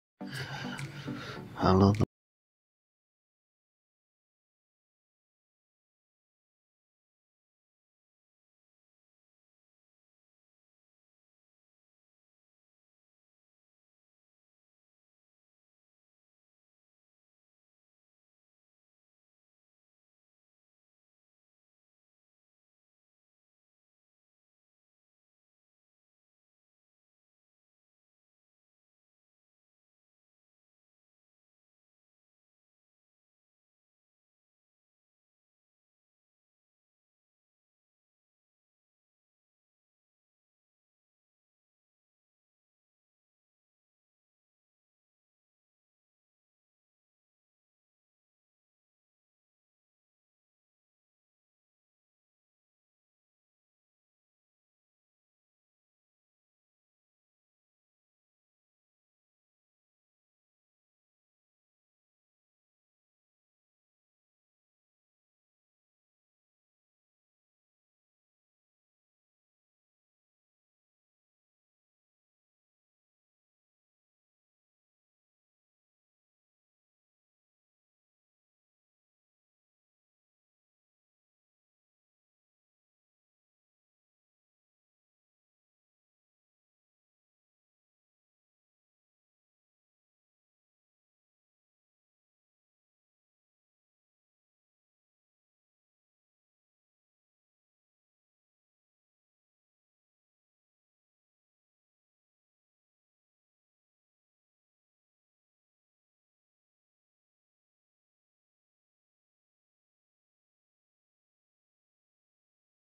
Grilled Chicken
Prosesnya Ayam Panggang Mudah Simpel dan hasilnya memukau mantul siap saji..